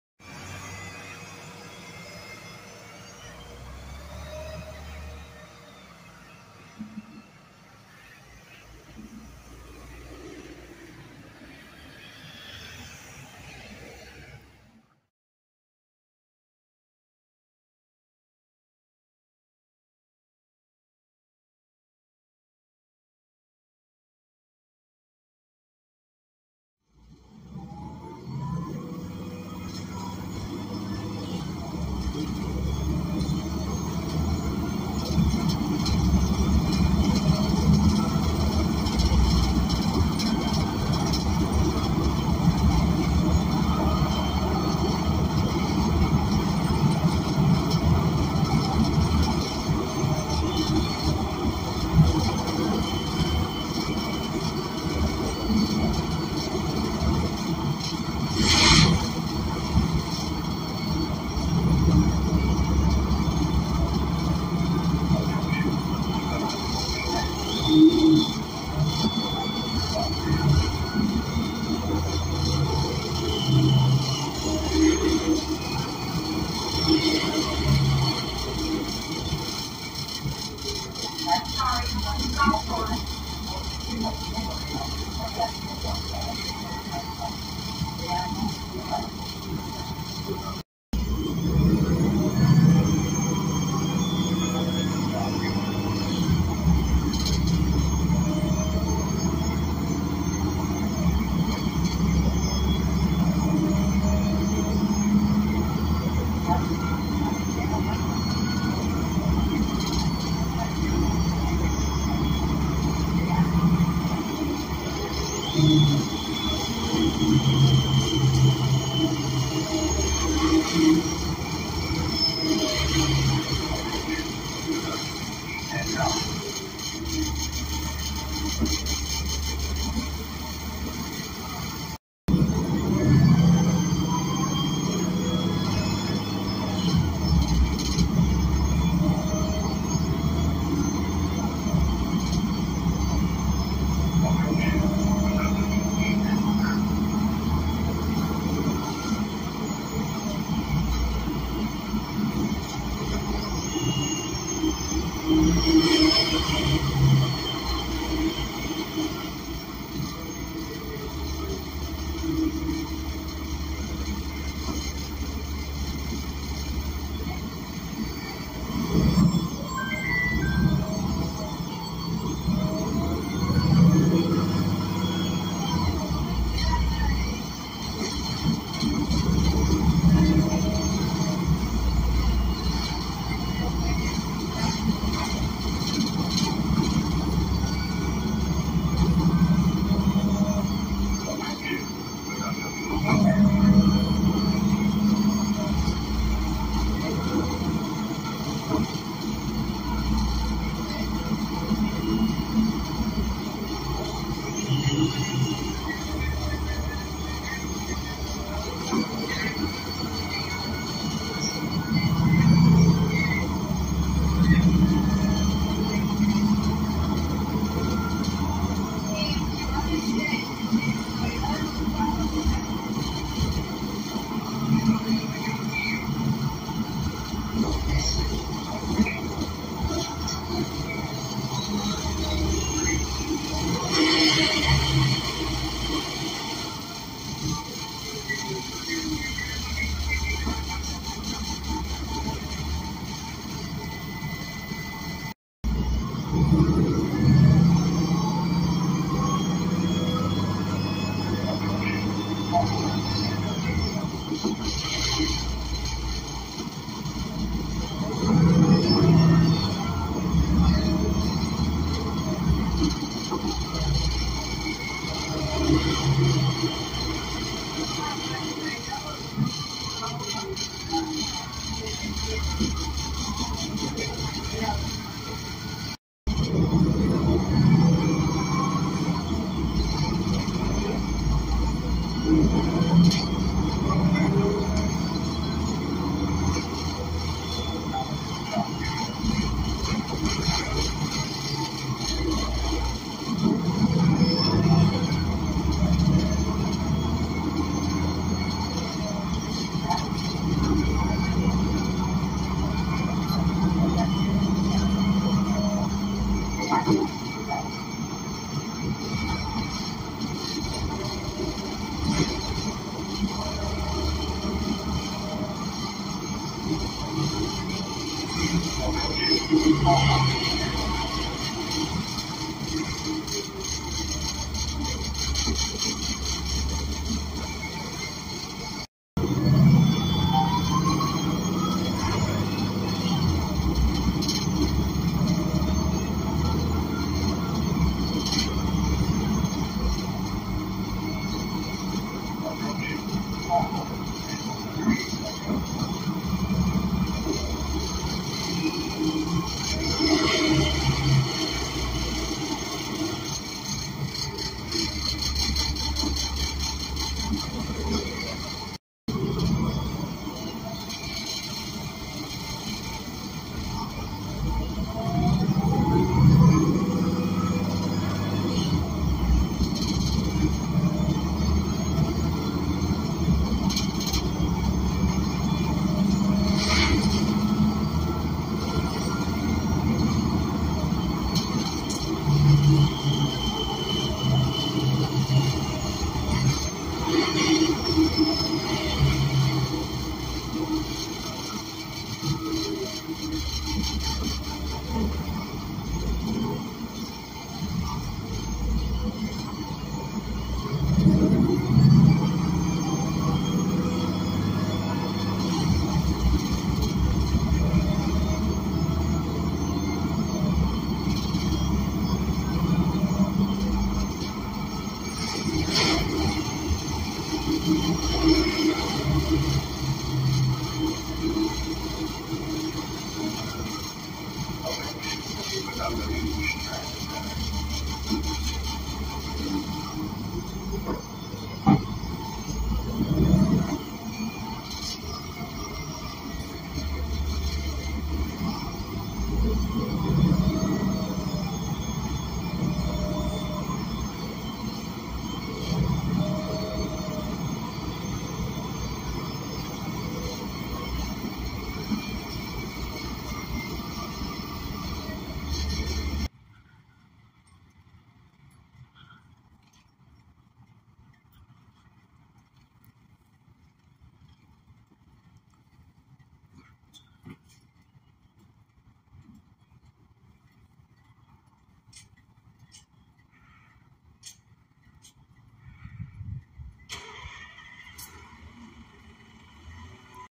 2020 LACMTA ENC Axess BRT 1798
I rode 1798 on the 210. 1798 was operated by Division 10 when released. In June 2020, it was transferred to Division 18.

Specs:
Cummins Westport L9N
Voith D864.6
Hanover Monochrome

Date: 12/10/23

This is my bus ride from El Camino College to Redondo Beach Transit Center.